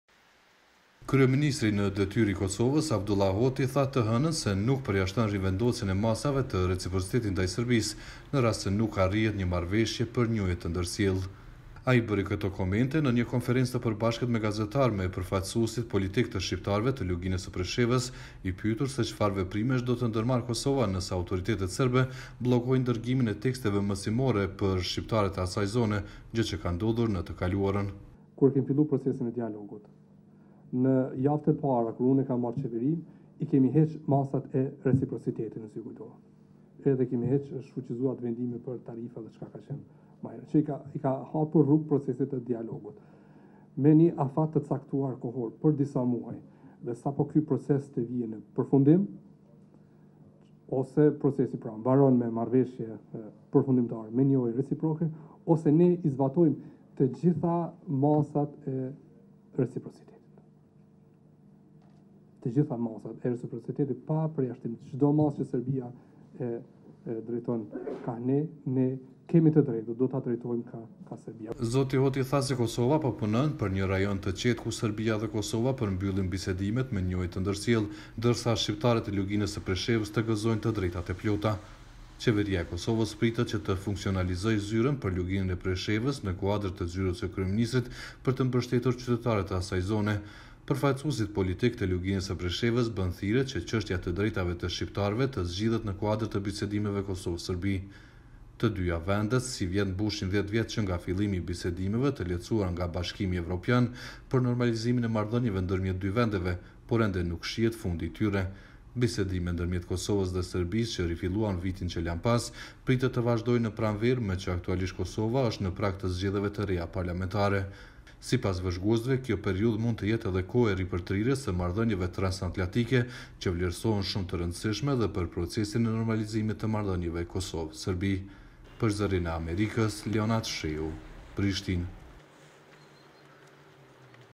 Hoti: Reciprocitet me Serbinë nëse nuk ka marrëveshje për njohje
Në Kosovë, kryeministri në detyrë, Avdullah Hoti, tha se nuk e përjashton mundësinë e rivendosjes së masave të reciprocitetit me Serbinë, në rast se nuk arrihet një marrëveshje për njohje të ndërsjellë Ai bëri këto komente pas takimit me përfaqësuesit e Luginës së Preshevës të cilët kërkuan që çështja e të drejtave të tyre të zgjidhet në kuadër të bisedimeve Kosovë – Serbi të lehtësuara nga Bashkimi Evropian.